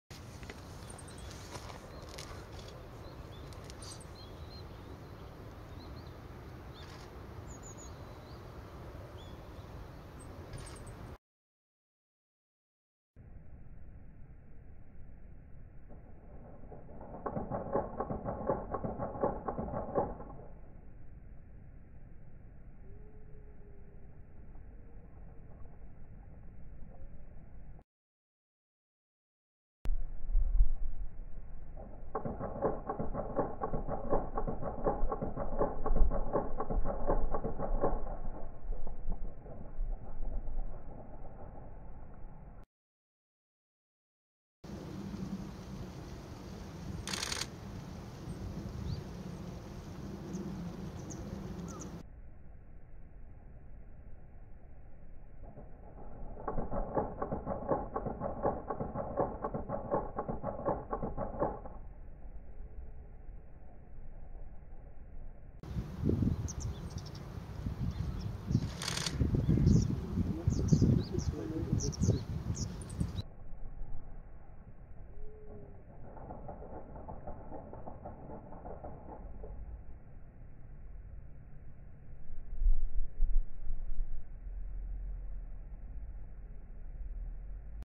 再びテリトリー争い（2017.11.30）
一旦はテリトリーが決まったかと思いましたが、再びバトル開始しました